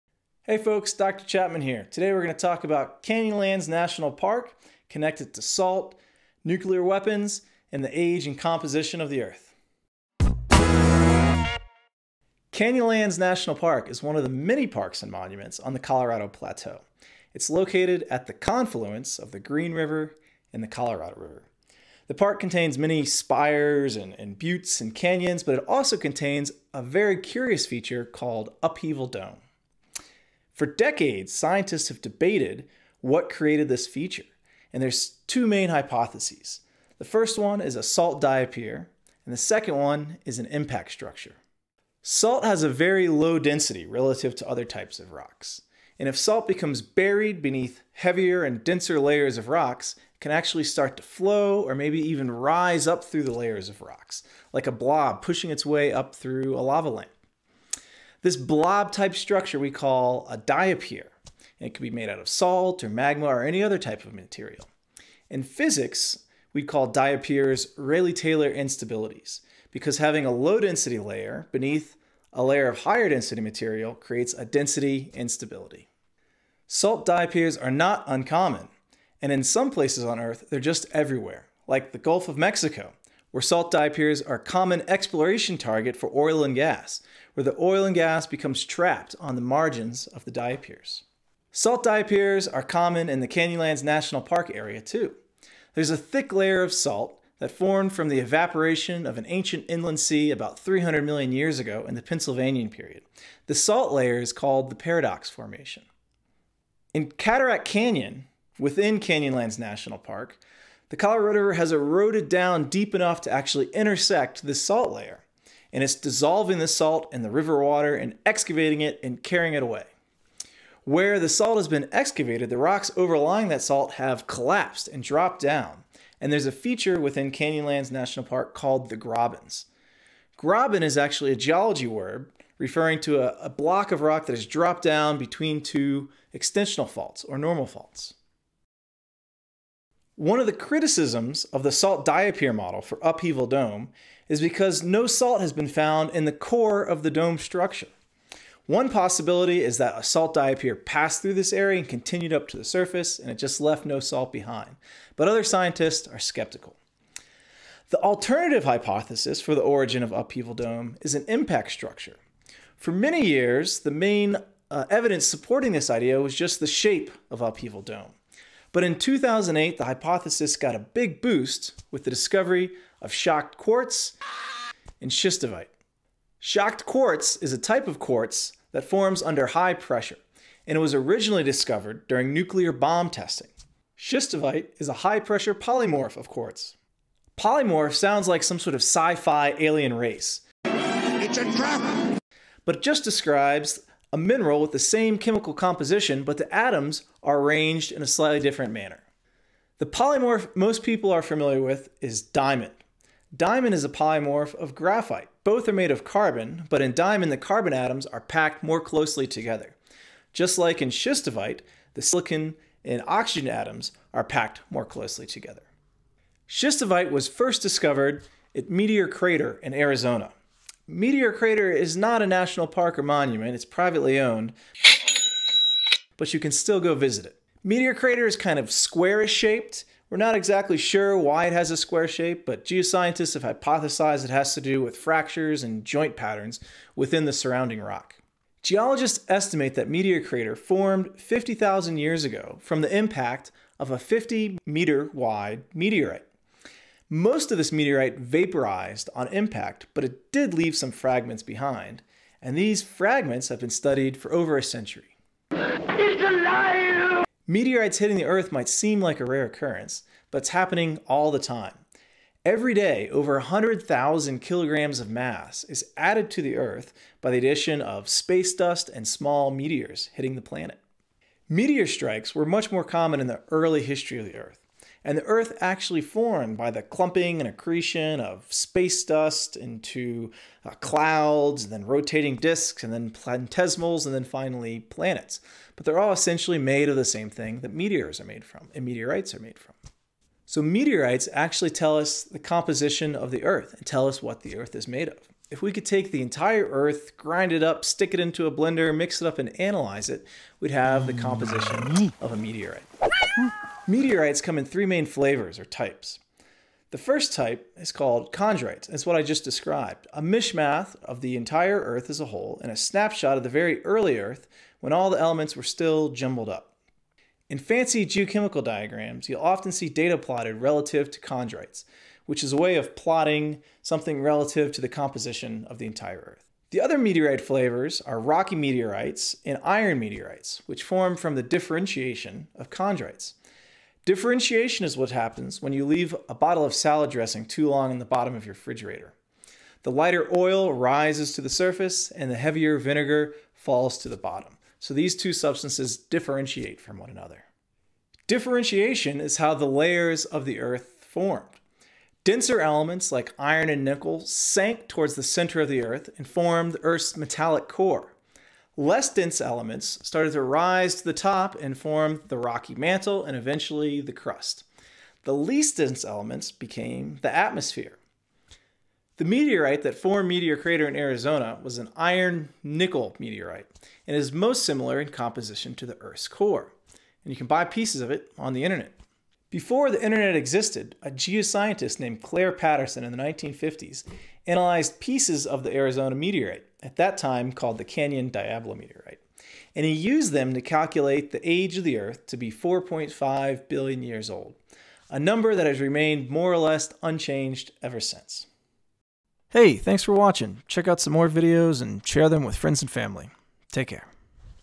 Geology of Canyonlands and the formation of the Earth
Learn about Upheaval Dome and the Grabens area in Canyonlands National Park, Meteor Crater in Arizona and how meteorites provide information about the age, composition, and layering of the Earth.